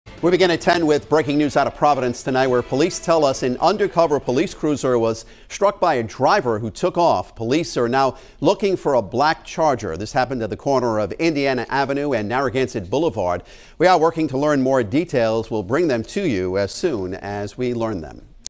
Undercover police cruiser hit by car in Providence
Police are searching for the driver who hit and damaged an undercover cruiser in the capital city Tuesday evening.